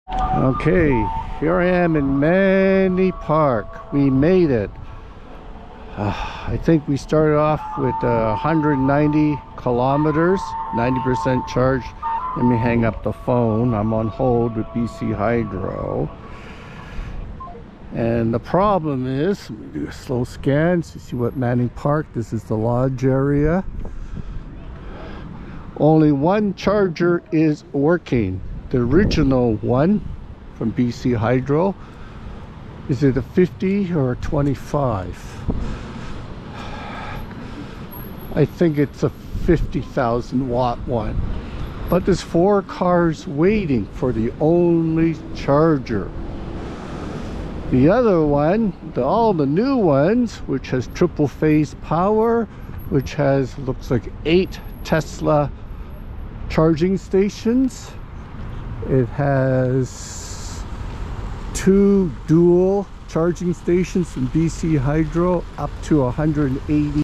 Electric Truck pulling a 22' trailer around British Columbia
I learned, for every 1 hour driving uphill, I must charge 1 hours.